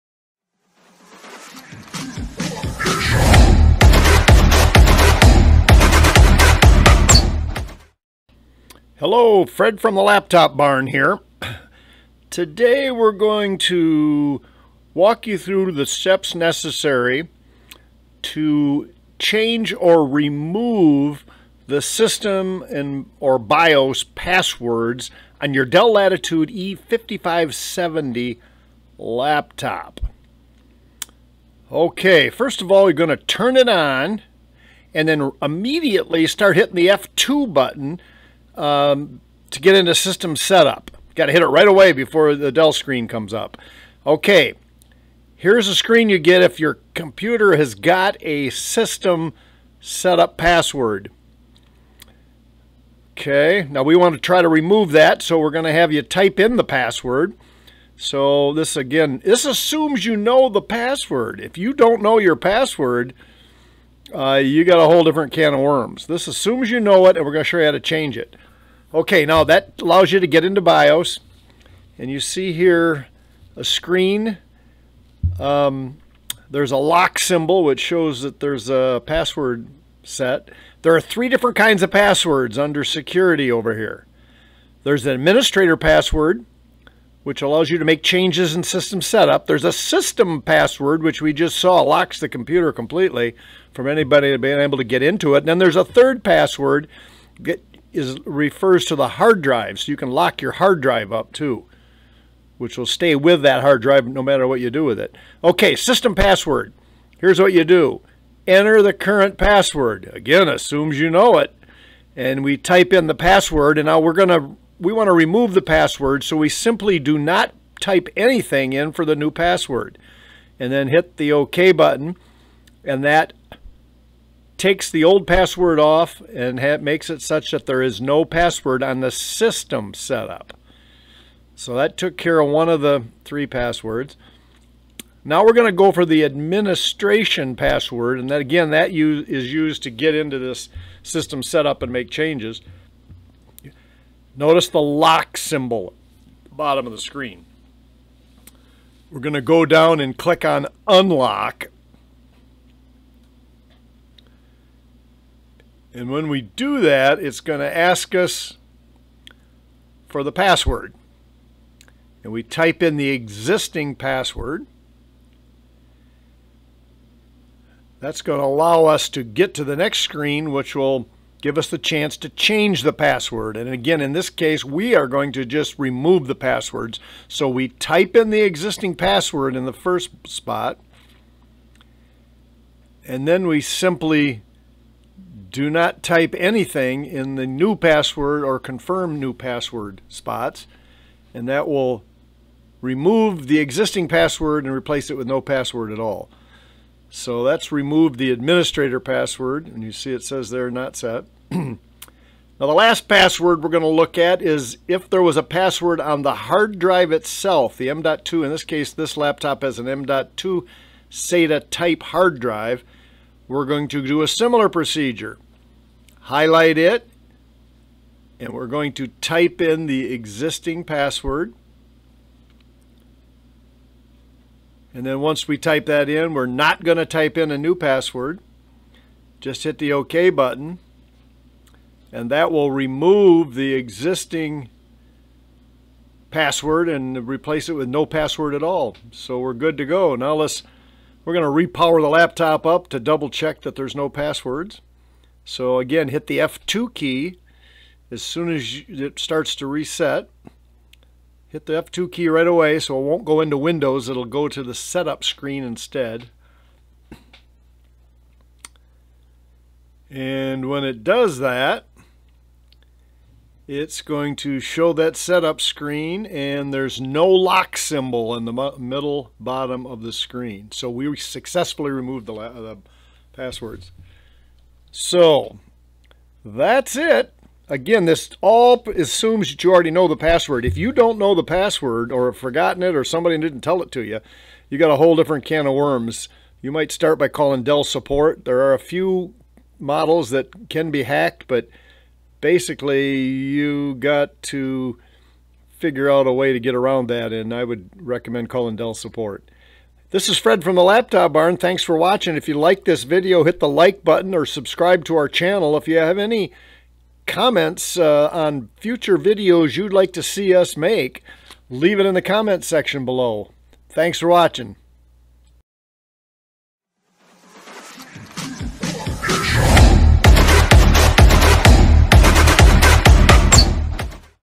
How To Remove the Passwords On A Dell Latitude E5570 Laptop
How To Remove or change The System, administrator, and hard drive Passwords On A Dell Latitude E5570 Model Laptop